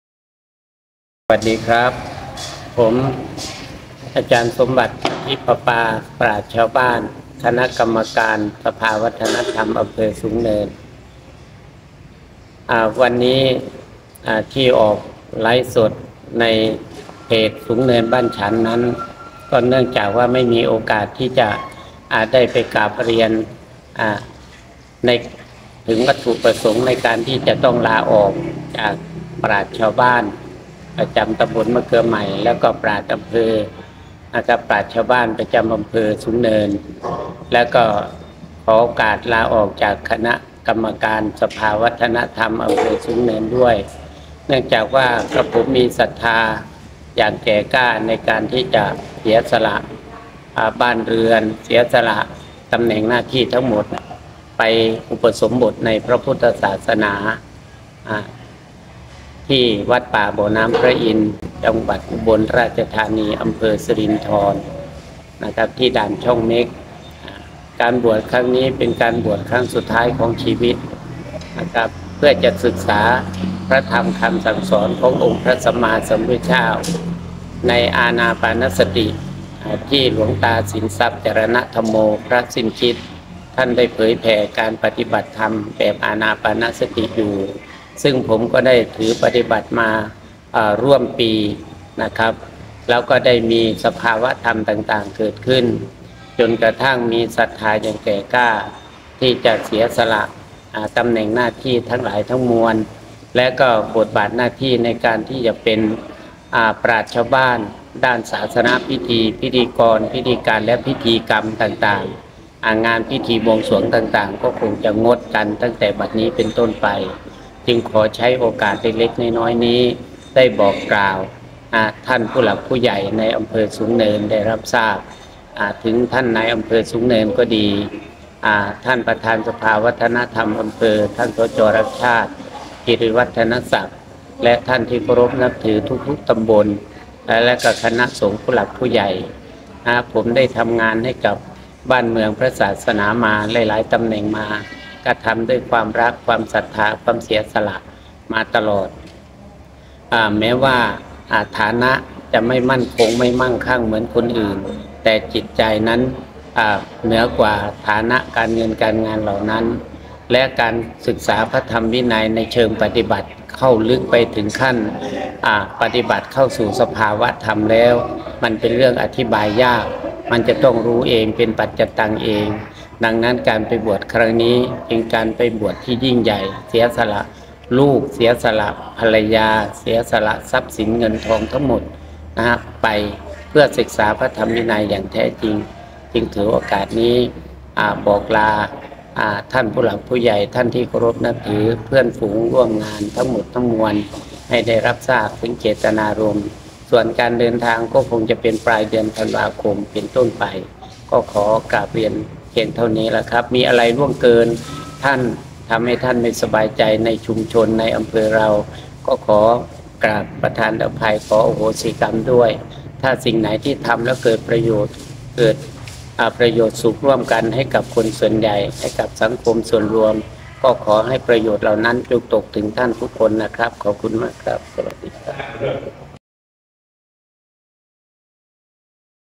อาจารย์สมบัติ ปธิปะปา ลาออกจากประธานสภาวัฒนธรรมฯ ปราชญ์ชาวบ้าน ไปบวชที่ วัดป่าบ่อน้ำพระอินทร์
อาจารย์สมบัติ ปธิปะปา ขอลาออกจากประธานสภาวัฒนธรรมตำบลมะเกลือใหม่ และปราชญ์ชาวบ้าน เพื่อลาบวช ครั้งสุดท้ายของชีวิต ที่ วัดป่าบ่อน้ำพระอินทร์
หลวงตาสินทรัพย์ หรือพระสิ้นคิด

อาจารย์สมบัติ ปธิปะปา ขอลาออกจากคณะกรรมการสภาวัฒนธรรมตำบลมะเกลือใหม่ และปราชญ์ชาวบ้าน เพื่อลาบวช ครั้งสุดท้ายของชีวิต ที่ วัดป่าบ่อน้ำพระอินทร์
กราบลา นายกฤษณธร เลิศสำโรง
นายอำเภอสูงเนิน นายรักชาติ กิริวัฒนศักดิ์ (สจ.ดุ่ย) ประธานองค์การบริหารส่วนจังหวัดนครราชสีมา ประธานสภาวัฒนธรรมอำเภอสูงเนิน และผู้ใหญ่ที่นับถือในอำเภอสูงเนินทุกท่าน กราบเรียนเชิญกัลยาณมิตร กัลยาณธรรม ญาติธรรม ท่านที่เคารพนับถือทุกท่าน

🍁ร่วมอนุโมทนาบุญพิธีตัดผม โกนผมนาค🍁
อาจารย์สมบัติ ประธิ ปะปา (ปราชญ์ชาวบ้าน)

เพื่อเตรียมตัวไปอุปสมบท ที่วัดป่าบ่อน้ำพระอินทร์ บ้านเรือนสีดา ต.ช่องเม็ก อ. สิรินธร จ.อุบลราชธานี (หลวงตาสิ้นคิด) *บวชไม่มีกำหนด
ณ บ้านพรนิพพาน
(บ้านคุณซิ้ม คุณภูษิต) หมู่ 5 ตำบลมะเกลือใหม่ อ.สูงเนิน จ.นครราชสีมา
(ก่อนถึงวัดใหม่สันติ 1 กม.)
🍀กำหนดการ
วันอาทิตย์ที่ 24 ธันวาคม พ.ศ 2566 (ขึ้น 12 ค่ำเดือน 1)
 เวลา 9.30 น. ภิกษุสงฆ์ถึงบ้านถึงบ้านพรนิพพาน
10.18 น. โกนผมนาค
11.00 น ถวายภัตตาหารเพลภิกษุสงฆ์
🍁และขอเชิญร่วมรับประทานอาหารโดยพร้อมเพียงกัน.

🍀กำหนดการเดินทาง ไปวัดป่าบ่อน้ำพระอินทร์ วันพุธที่ 27 ธันวาคม 2566
(ขึ้น 15 ค่ำเดือน 1)

🍁กรรมใดที่อาจารย์สมบัติ ได้เคยล่วงเกิน ท่านทั้งหลาย ด้วยกายกรรม วจีกรรม มโนกรรม
🍁ขอได้โปรดอโหสิกรรมนั้นให้แก่ผู้อุปสมบทด้วยเทอญ